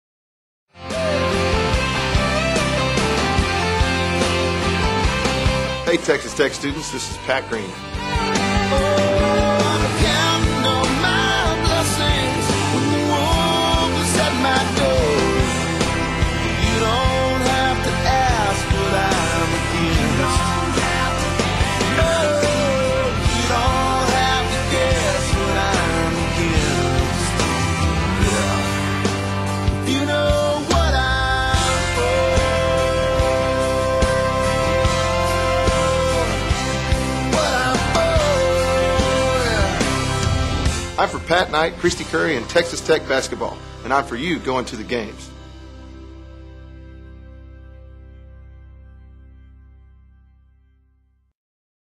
PAT GREEN SMALL 2.wmv
Pat Green wants you to be a part of 2009-10 Texas Tech Basketball!